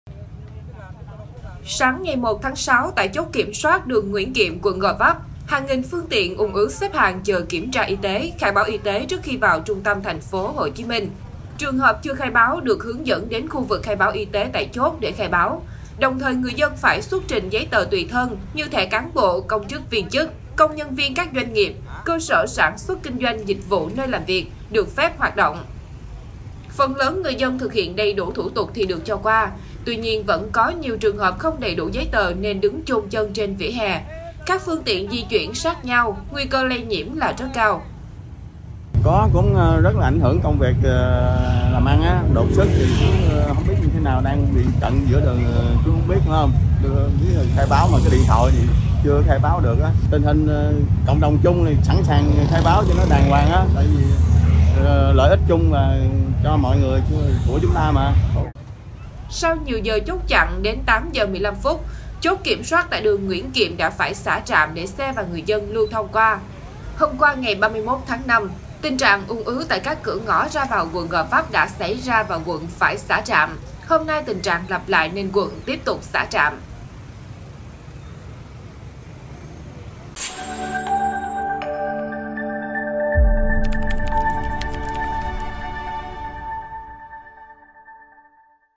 Lại xả trạm kiểm soát ra vào quận Gò Vấp do lượng phương tiện ùn ứ quá đông
Sáng 1-6, tại chốt kiểm soát đường Nguyễn Kiệm (quận Gò Vấp), hàng nghìn phương tiện ùn ứ xếp hàng chờ kiểm tra y tế, khai báo y tế trước khi vào trung tâm TPHCM. Đến 8 giờ 15 phút, lực lượng chức năng đã phải xả trạm để xe và người dân lưu thông qua.